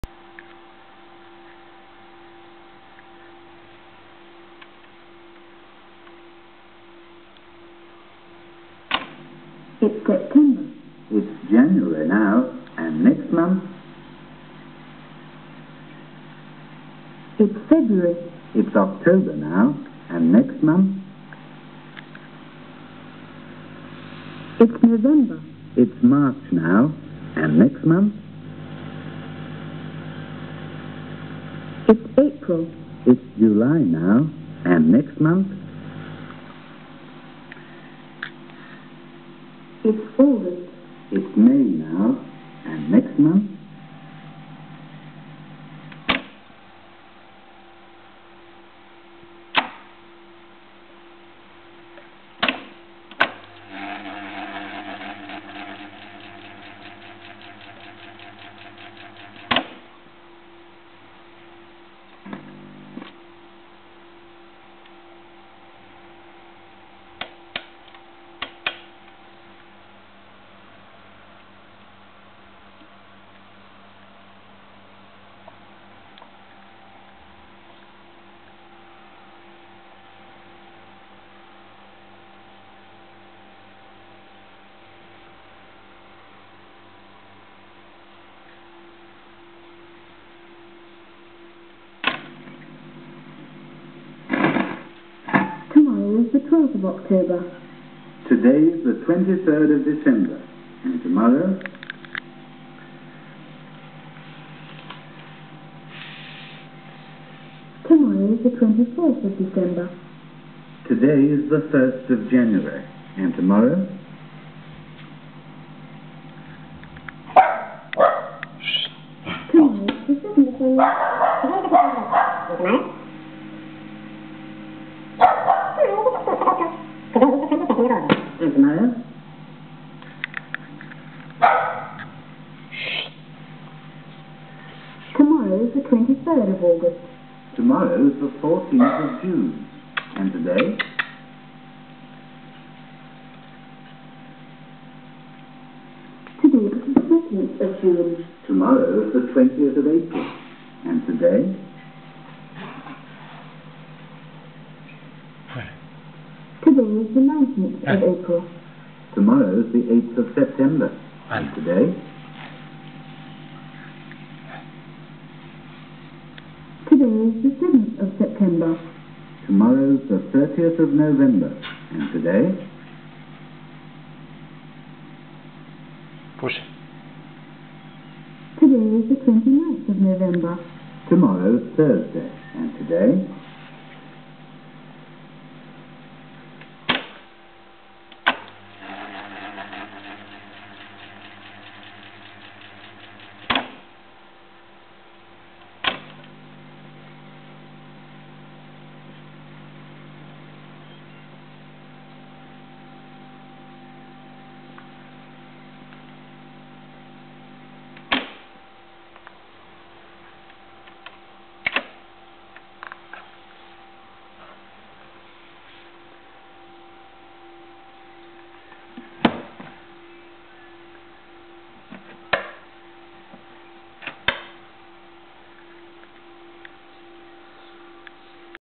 P.A.M labs restoring RFT SMARAGD ΡΕΕL TO REEL
An iconic reel to reel from the RFT. Few mentioned the inside quality , outside is amazing anyway, Speaking for the construction one is for sure: built like a panzer , reminds a military construction. The recorder was received in good condition except ...a broken bakelit and a damaged wood case. more over needed work on the motor , re-lubed all moving parts and clean the fuse holders. Of course there were many points here and there needed restoration , too. Now is just a perfect example!